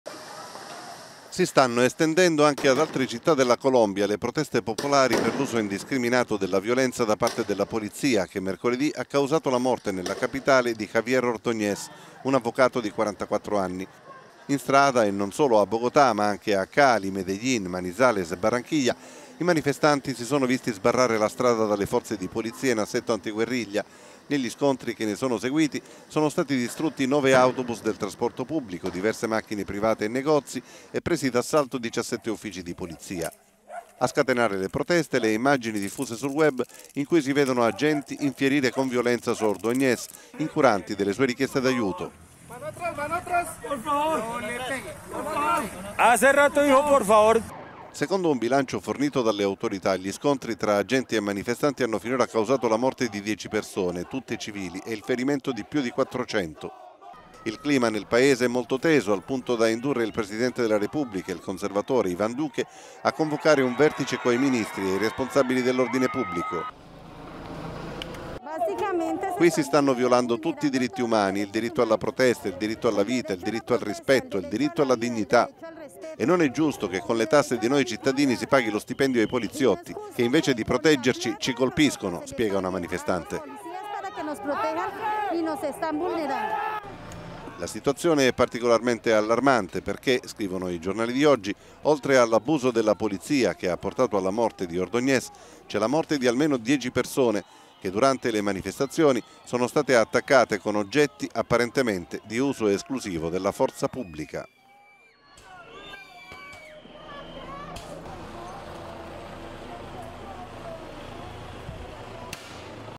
Colombia, manifestazioni contro la violenza della polizia
Scontri in tutto il paese dopo l'uccisione durante un arresto di un avvocato di 44 anni. Finora il bilancio è di 11 morti e oltre 400 ferit